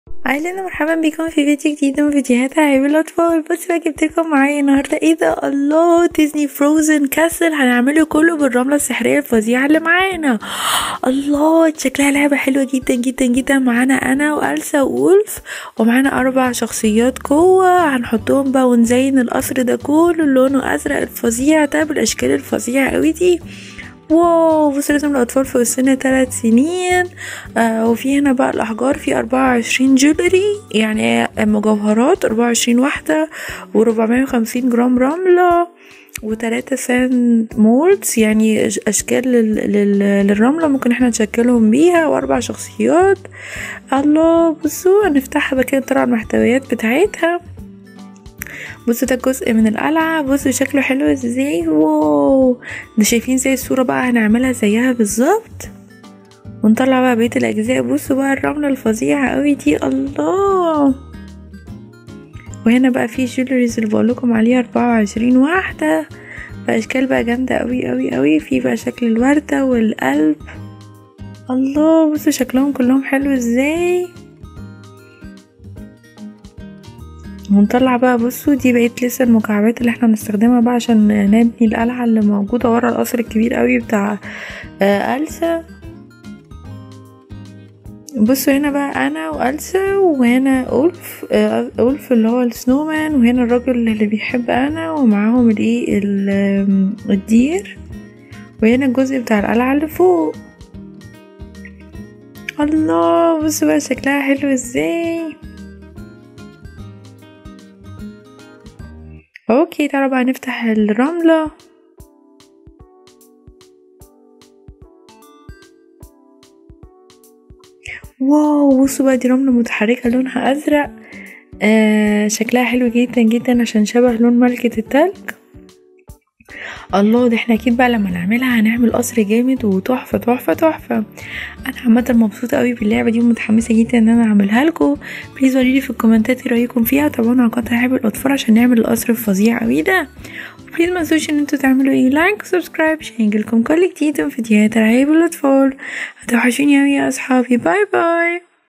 قلعة الجليد من السا ملكة الثلج عن بناء القلعة الرملية مع بلورات كبيرة
يمكن ايضا ان تكون هذة القنوات مفيدة لكم:
يمكن ايضا ان تكون هذة القوائم مفيدة لكم: